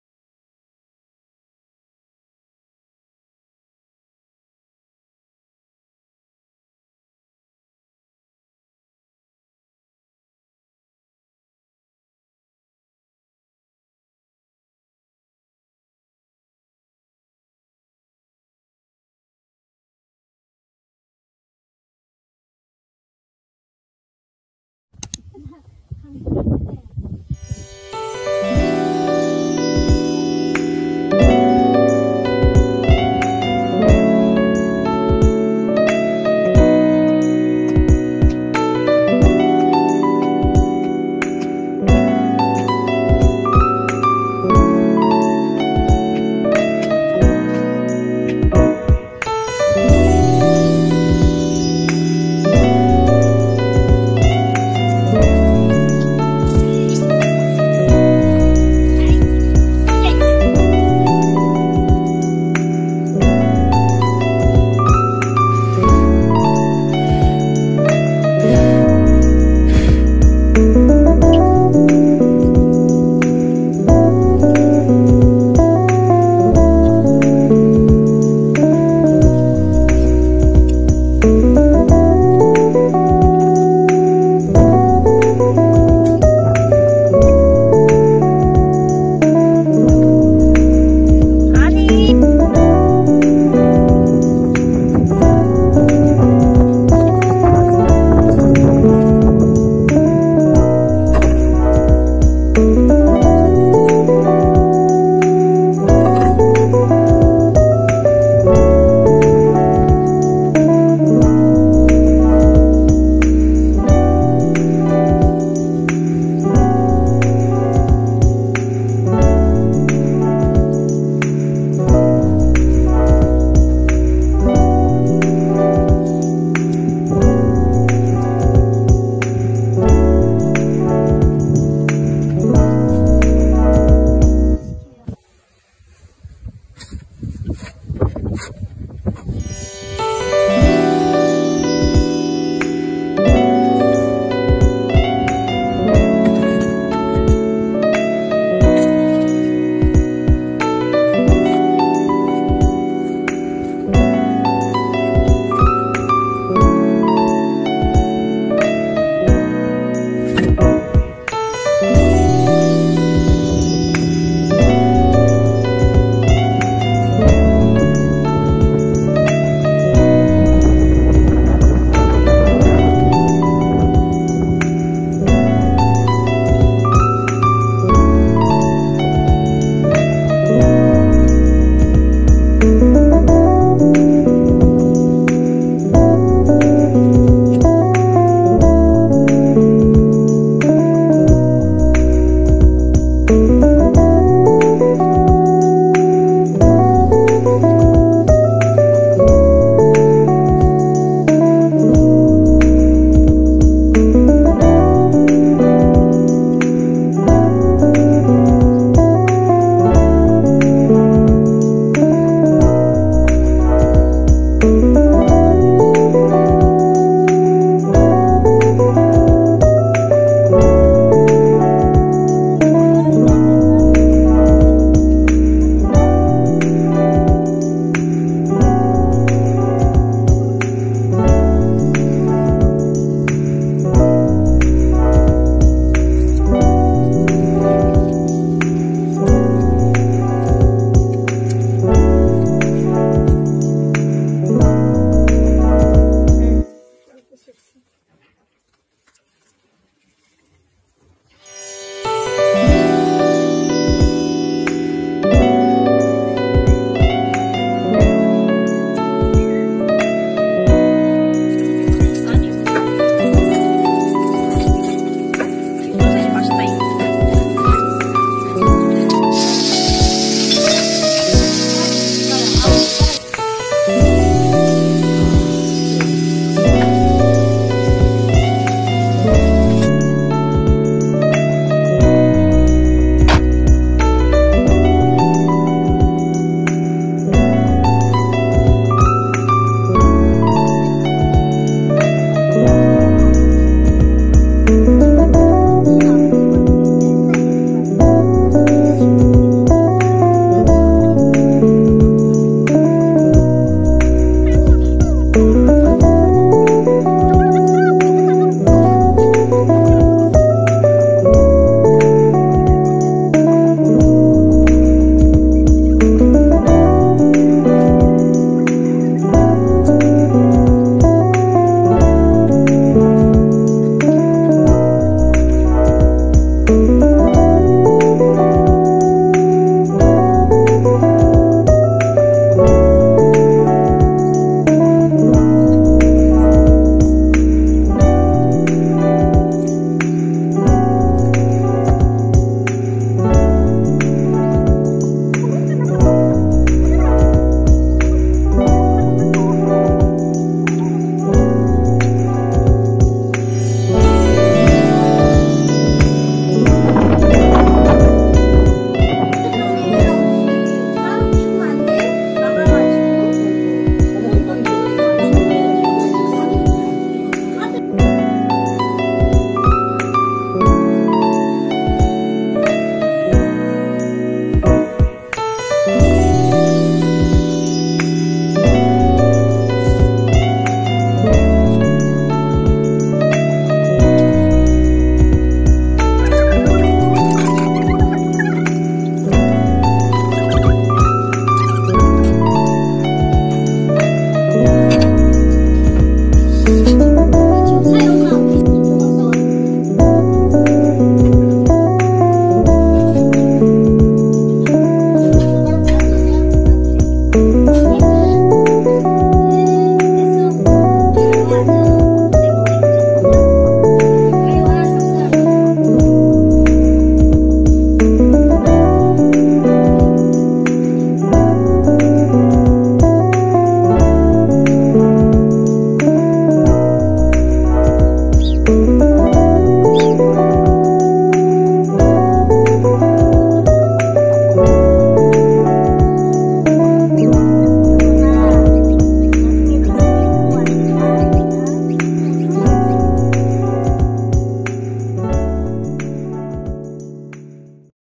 𝐯𝐥𝐨𝐠 | 20대 시골소녀 브이로그 | 노동으로 살아남기 | 시골 자연먹방 | My rural life
하이하이 안녕하세요
오늘은 처음으로 시골브이로그를 들고 왔는데요
계속 해외랑 다른 곳들만 올리니 스스로 너무 사대주의 같아서 시골일상도 만들어봤어요

저의 시골일상을 함께해주시죠

Please enjoy the video of my daily life in the countryside.
I will continue to frequently upload countryside videos.

私の田舎の日常映像を一緒に楽しんでください
私は今後も田舎のビデオを頻繁にアップロードします。
そして海外旅行のビデオもアップロードするので、私を購読してください